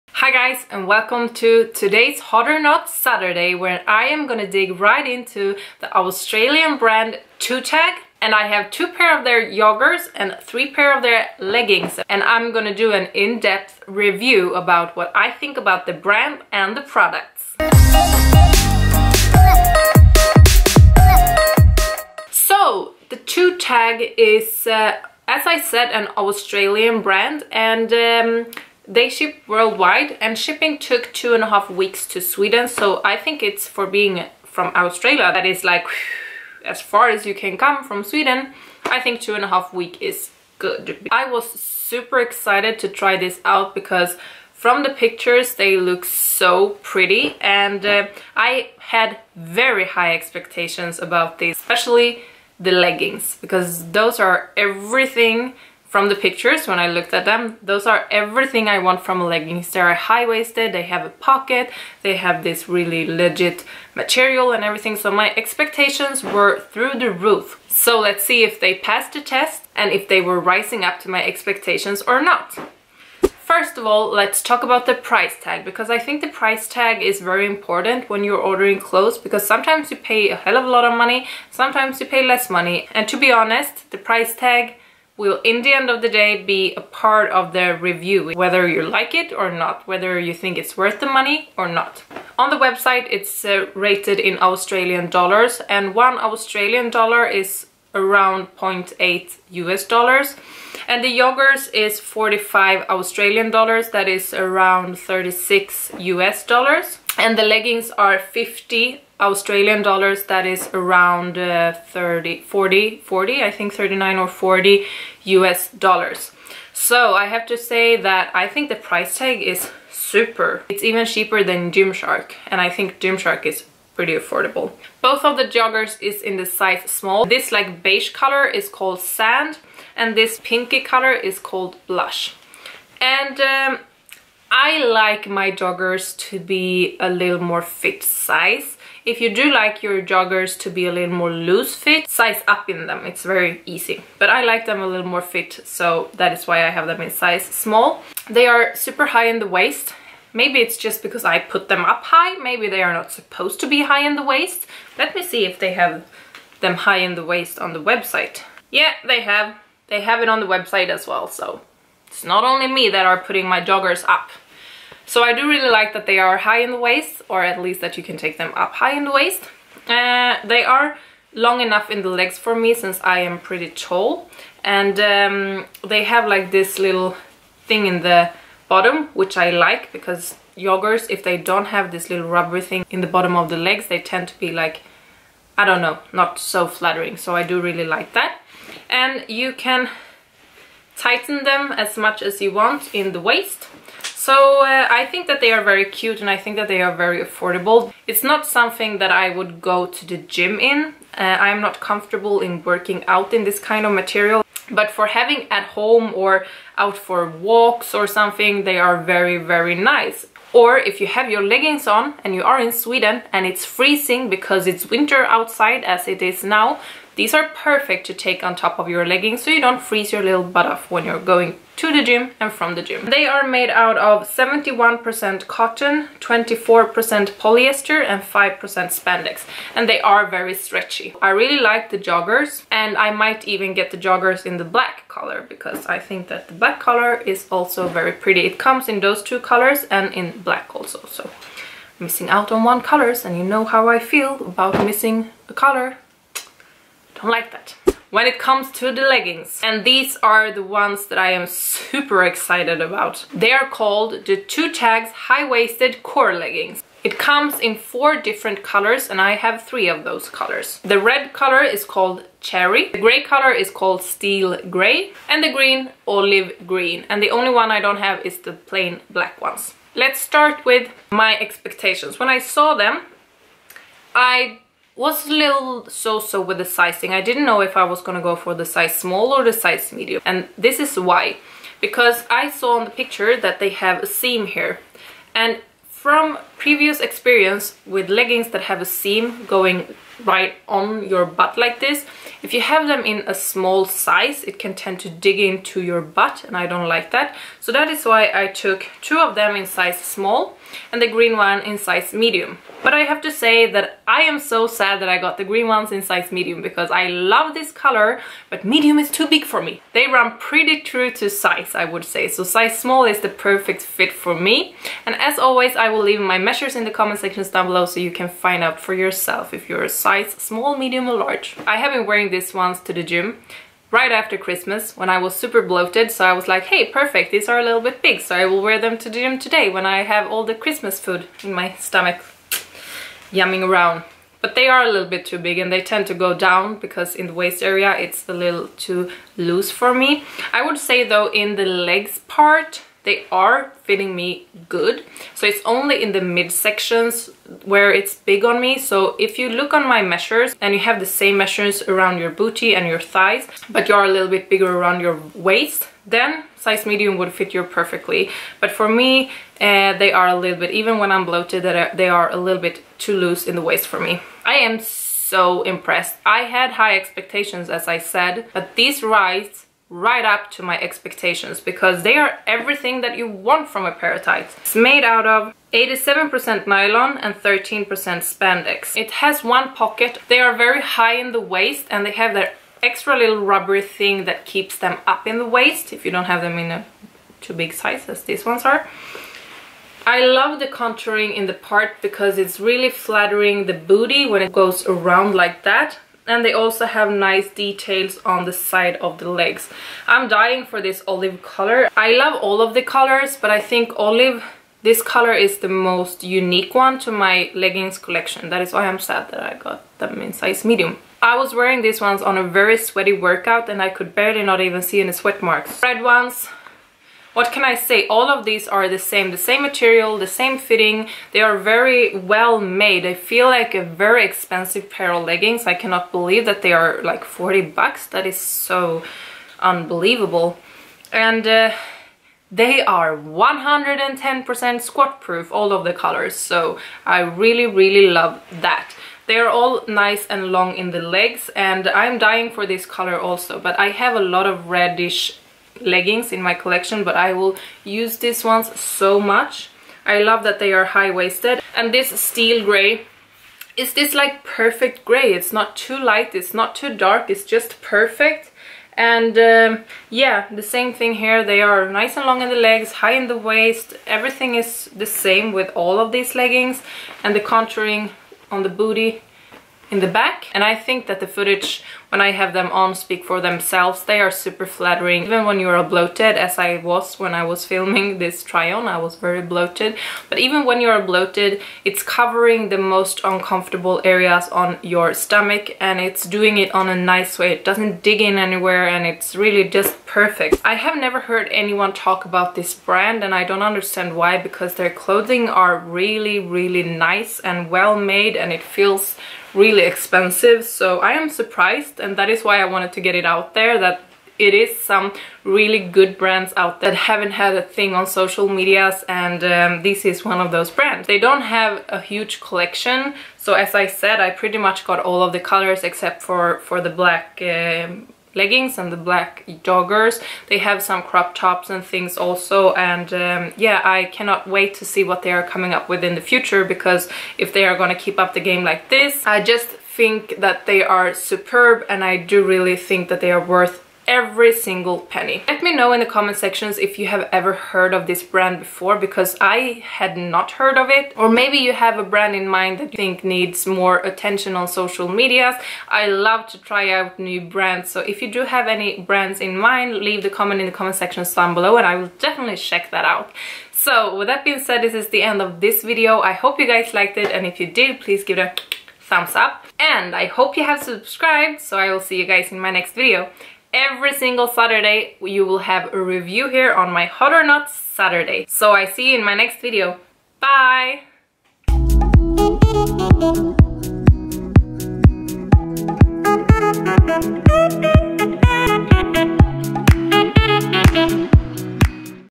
Twotags Review | New affordable favorite brand? | Hot or Not Saturday
Twotags leggings & joggers review.
Full Review, try o and honest opinion of the brand Twotags.

Underwear I wear in try on (I wear seamless low raise super thin in nude size M:
→ Bubblelime Sports Thongs:

My measurements:
Waist: 66cm
Tummy: 79cm (5 cm under belly button)
Hips: 98cm
Chest: 92cm
Under chest: 68 cm (for bra size guide)

I am 178cm (5’85) tall

Sens I am also a beauty lover I also have a Beauty Channel here on Youtube, Go and check it out=)